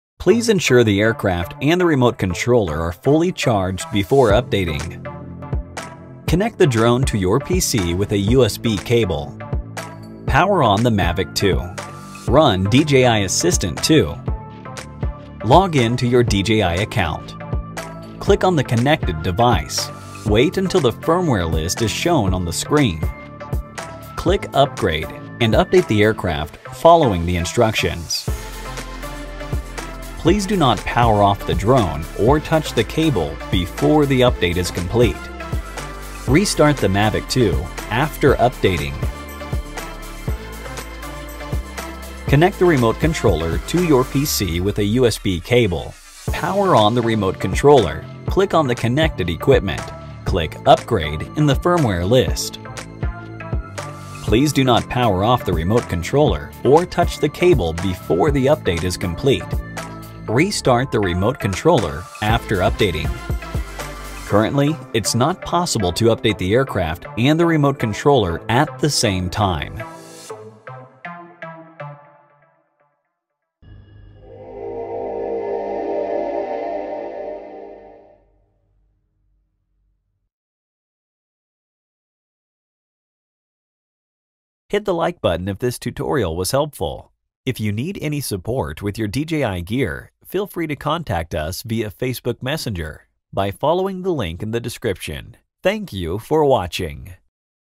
DJI Mavic 2 - aktualizace firmware přes PC (CZ titulky)
Podívejte se, jak se provádí aktualizace firmware za pomocí programu DJI Assistent.